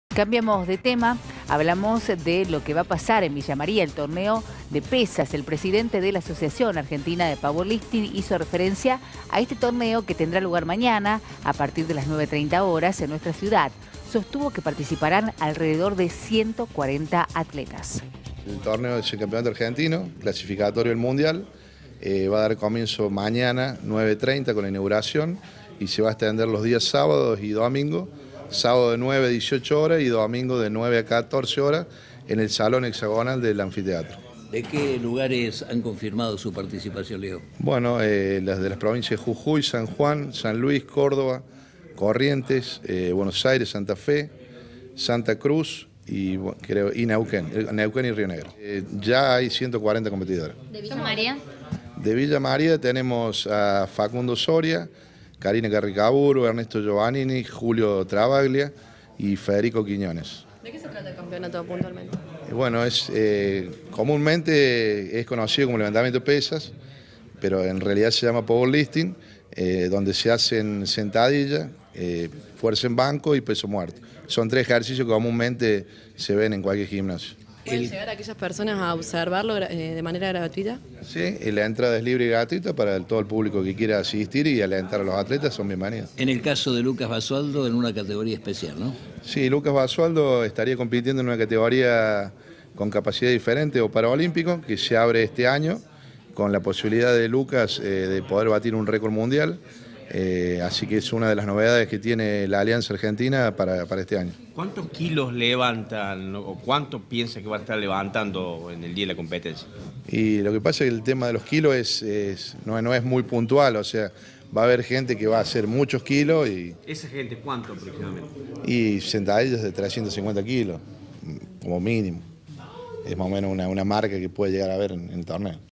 Torneo nacional de powerlifting
El presidente de la Asociación Argentina de Powerlifting, Leonardo Cavaglia, hizo referencia al torneo que tendrá lugar el fin de semana.